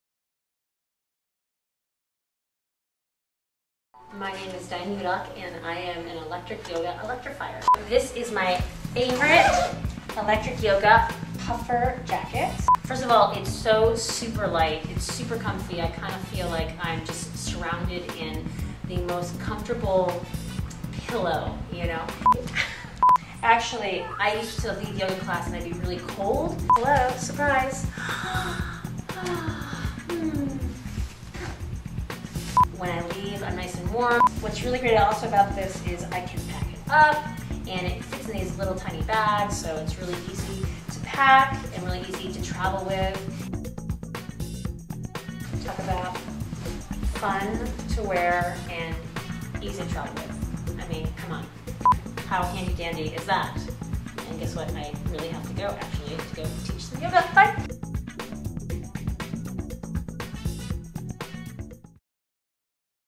Yogi Diane Hudock on Electric Yoga by Michele Bohbot
Electric Yoga Electrifier, Diane Hudock touches on her love for our AWESOME
Ultra-Light, Packable Jacket...